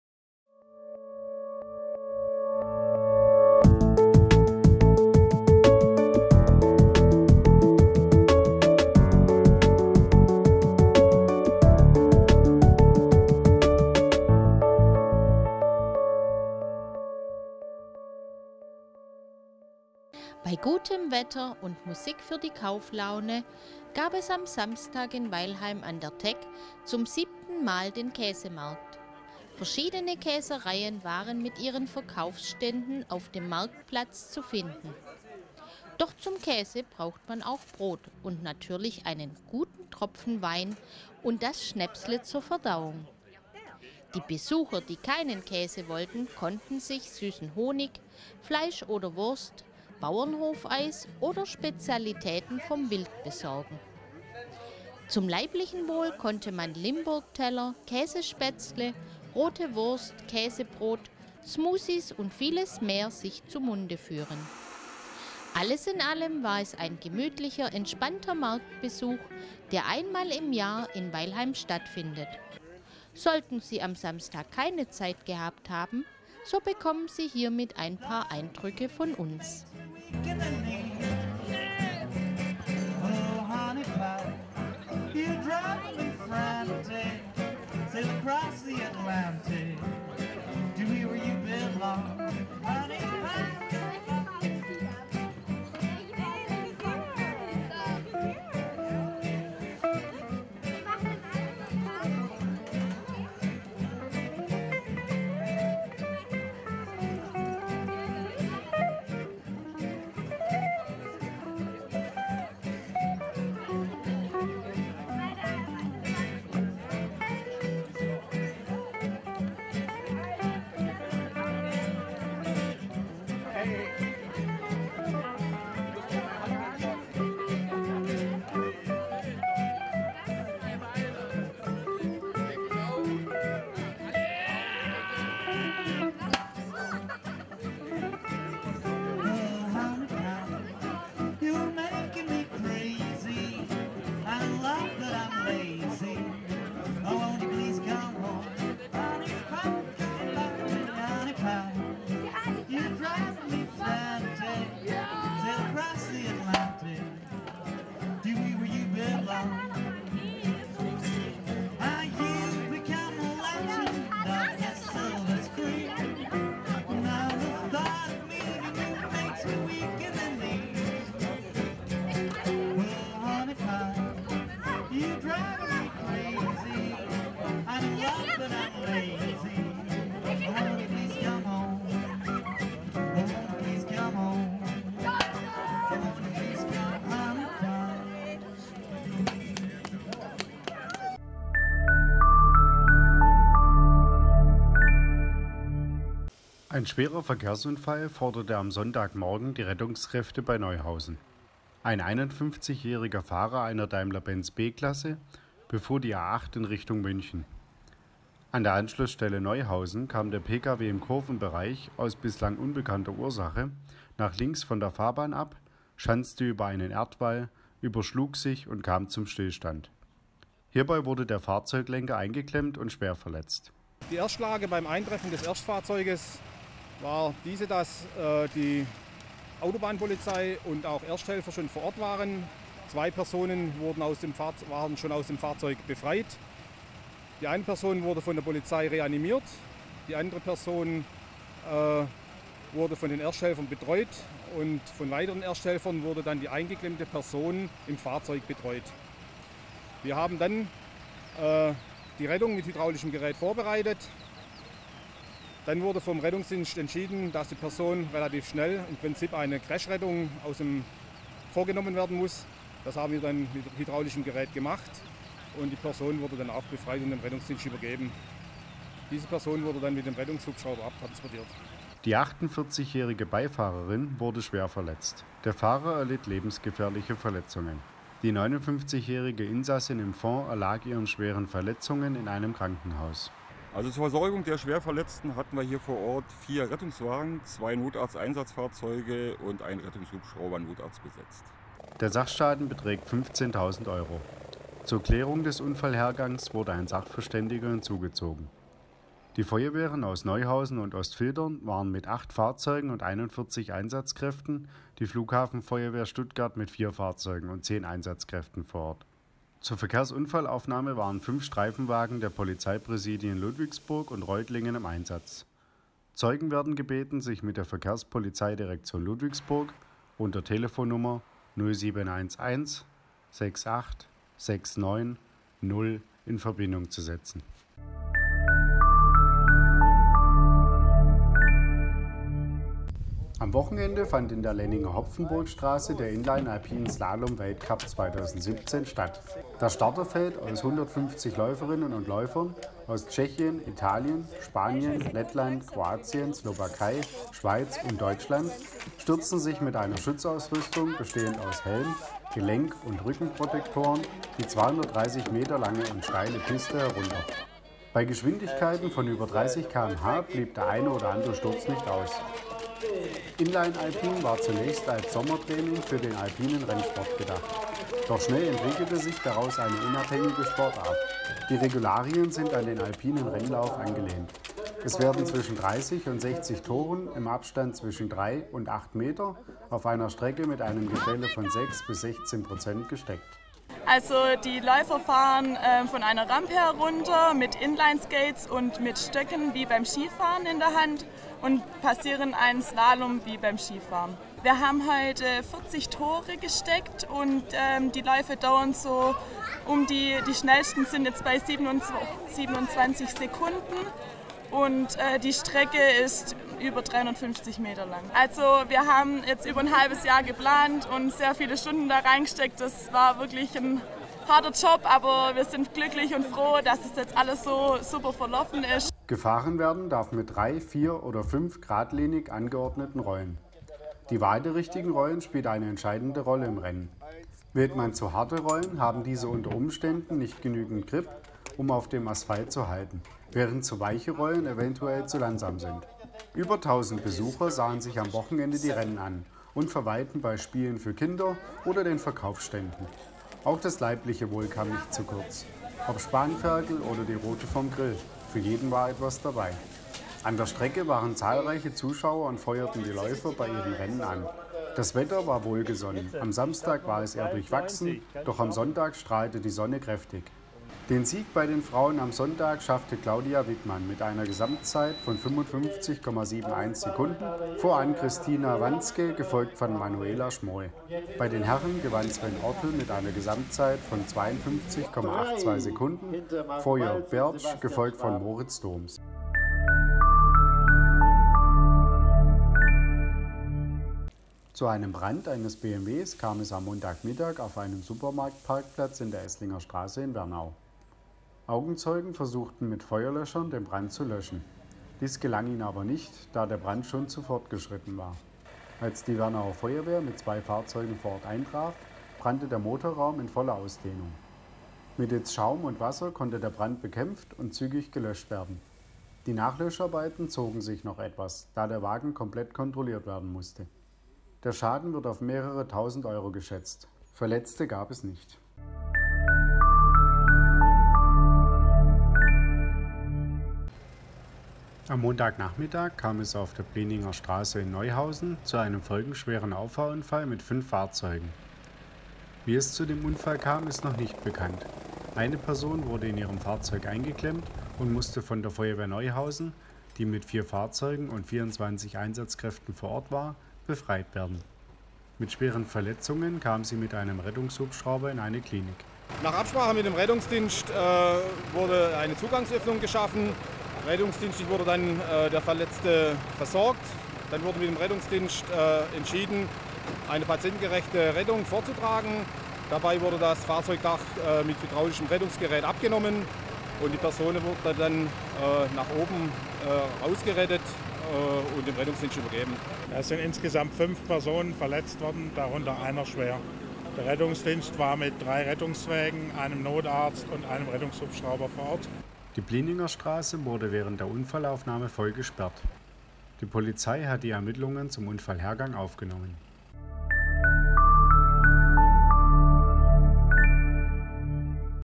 22.05.2017 – Die Meldungen des Tages
Unsere Themen vom Tag:
- 7. Weilheimer Käsemarkt
- Verkehrsunfall mit Fahrzeugueberschlag auf der A8 bei Neuhausen
- Alpin Slalom Weltcup 2017 in Lenningen
- Pkw brannte auf Supermarktparkplatz in Wernau
- Schwerer Auffahrunfall fordert 5 Verletzte bei Neuhausen